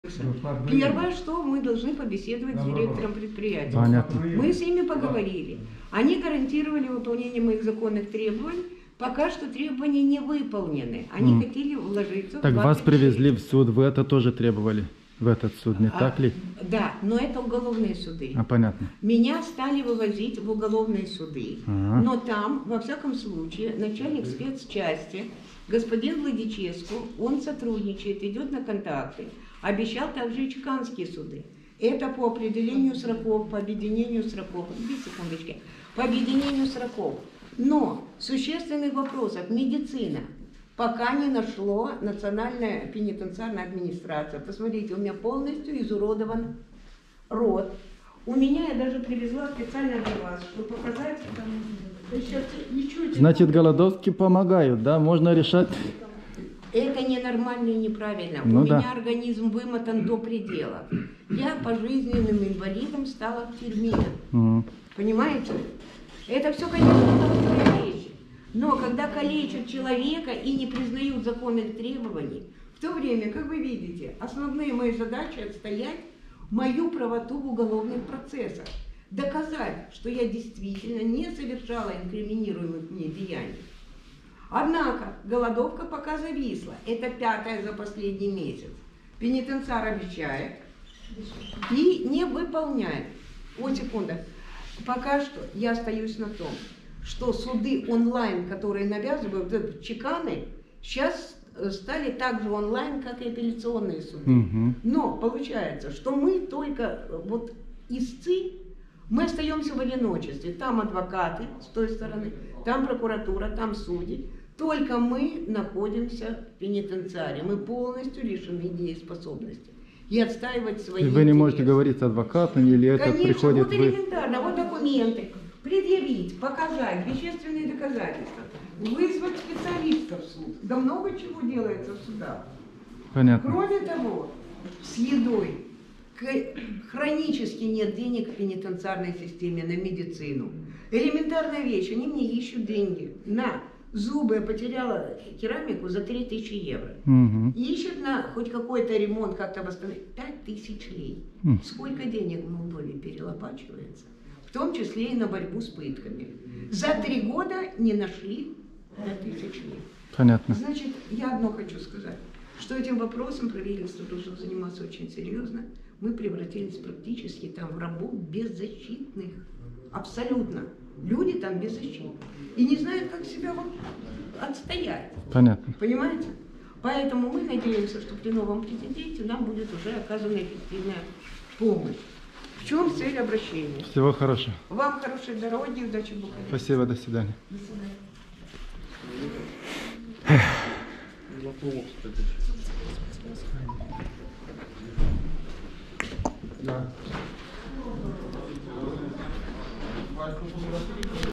Face greva foamei ca să revendice drepturi după gratii (ru) - Curaj.TV
Izabela Timofeeva e întemnițată de peste 6 ani, dar procesele de judecată continuă non-stop. Femeia protestează împotriva condițiilor inumane de detenție, face greva foamei ori-de-cîte-ori vrea să revendice un drept elementar